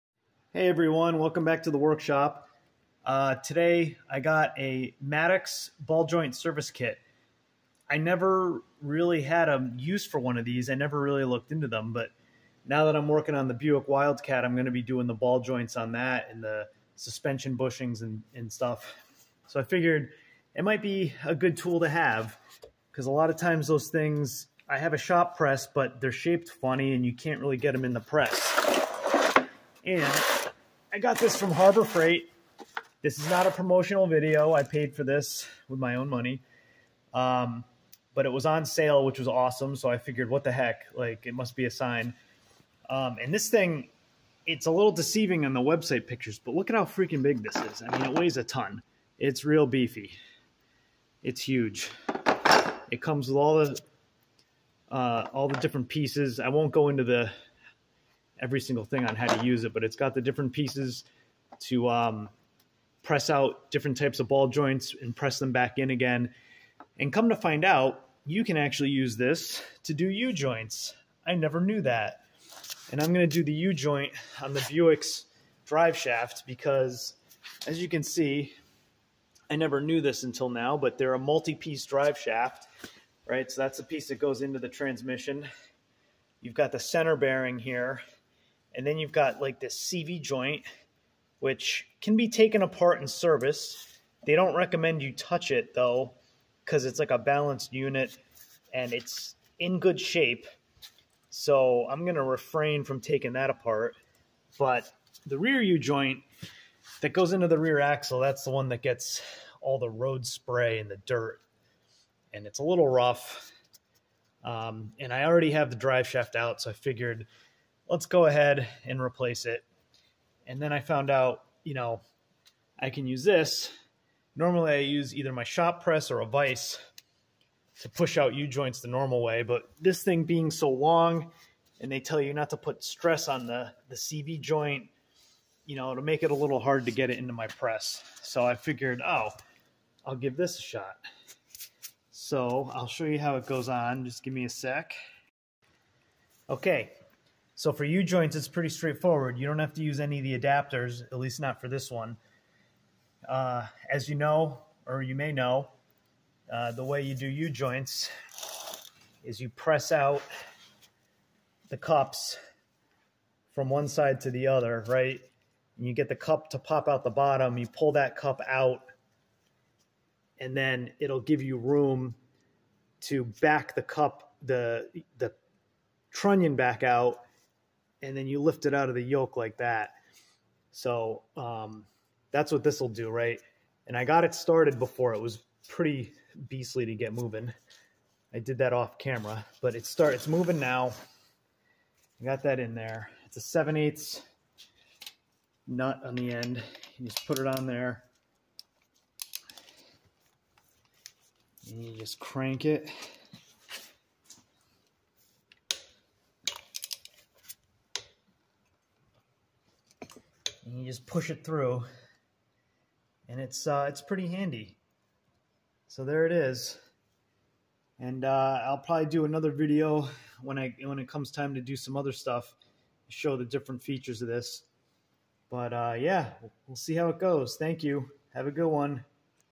Harbor Freight Ball Joint press to remove a U-joint
I just recently learned you can press out Universal Joints using a ball joint press.  I was looking at getting a ball joint press for working on the Buick Wildcat.  It came in handy especially with the Wildcat's long multi piece drive shaft.  Not a promotional video, I purchased the Maddox ball joint press when I saw a coupon from @harborfreight